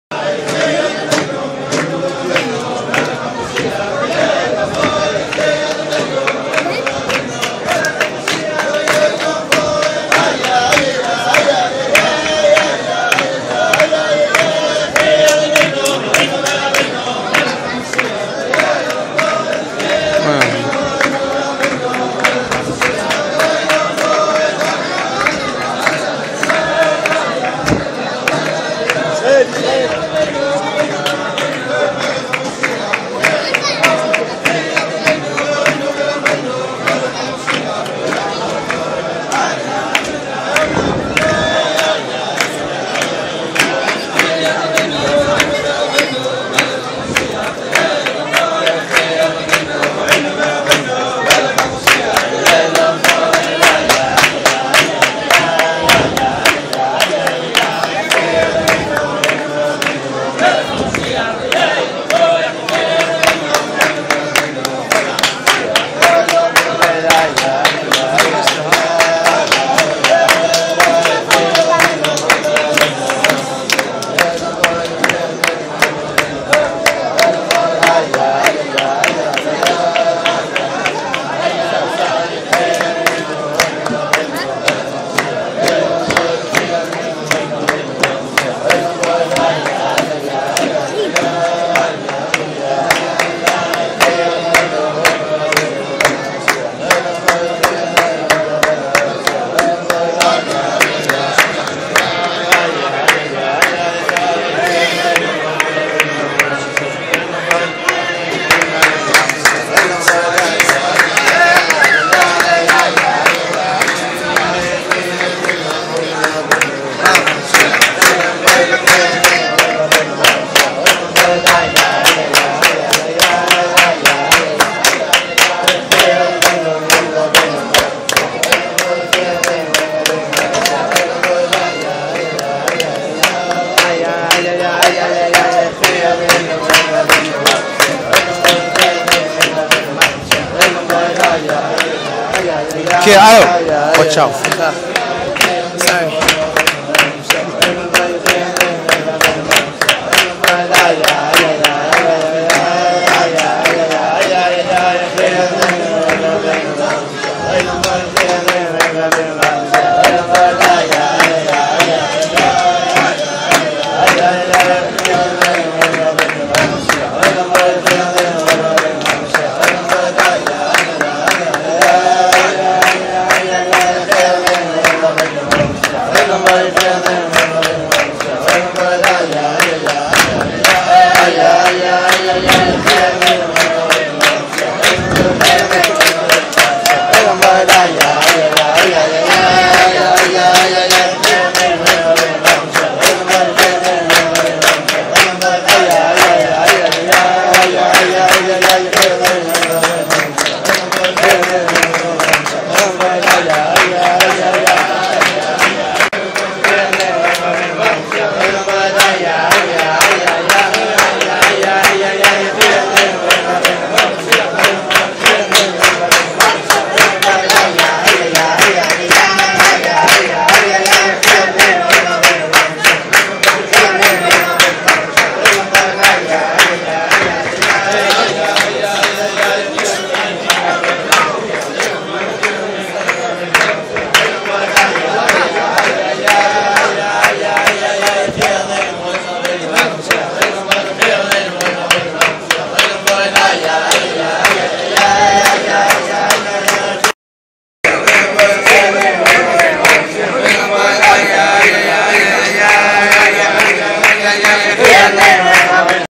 חלוקת דולרים להוספה בצדקה 2013-12-01 כח כסלו תשע"ד
חלוקת דולרים ע"י הרבי שליט"א מלך המשיח, מרכז חב"ד העולמי 770 ברוקלין ניו יורק, יום א' כ"ח טבת תשע"ד, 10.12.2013
לקראת סוף הסרטון נראה גבאי בית הכנסת, הרב הולצברג מסתכל על חלוקת הדולרים עם ידיד, (הרב הולצברג הוא אבא של גבי הולצברג הי"ד שנרצח בפיגוע המפורסם בבומביי הודו). בסוף החלוקה ניגש גם הוא לקחת דולר להוספה בצדקה.